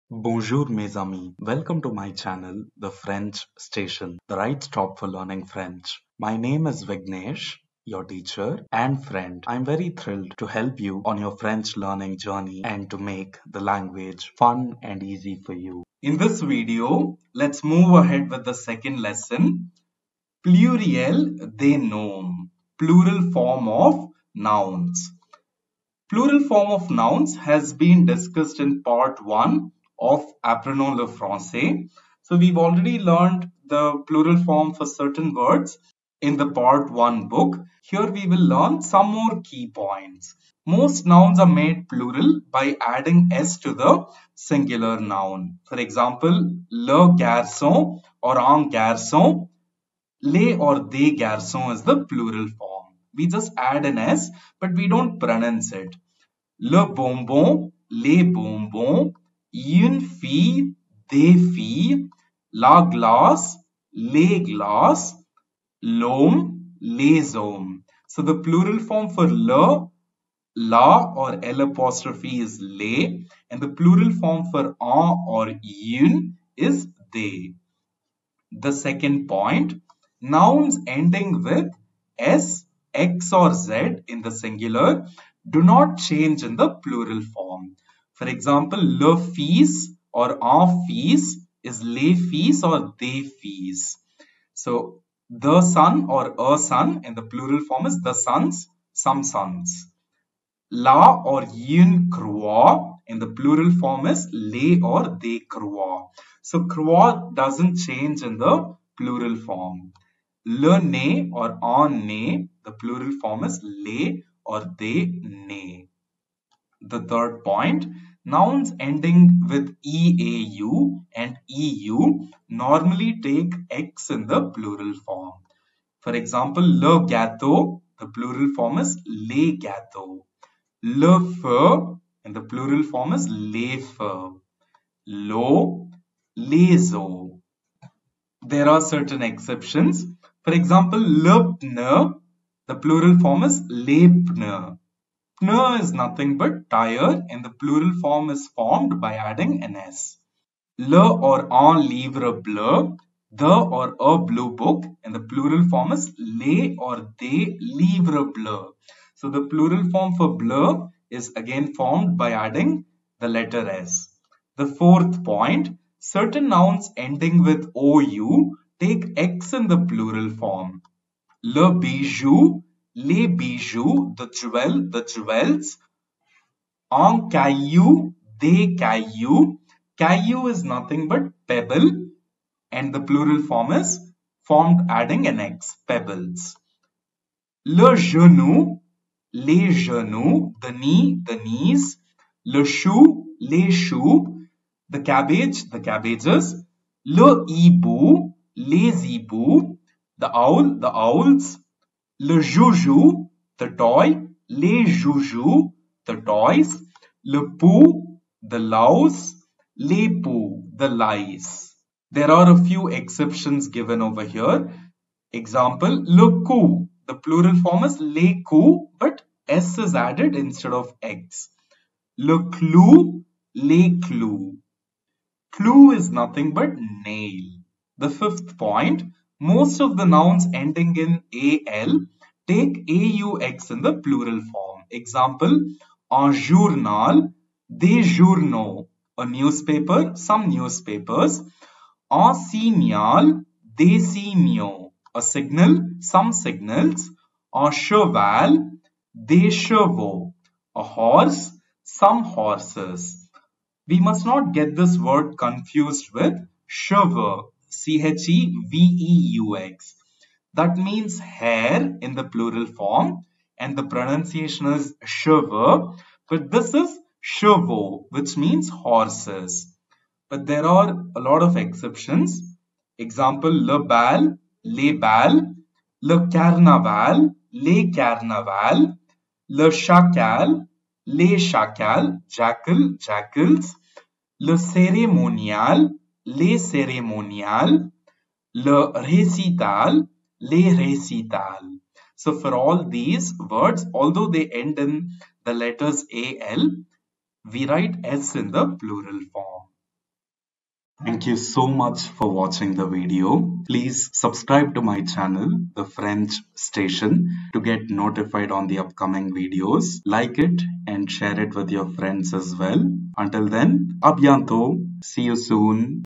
Apprenons le français-2, Méthode de français, Leçon-2, Part-2
This video covers the plural form of nouns given in lesson 2 from the book ALF-2.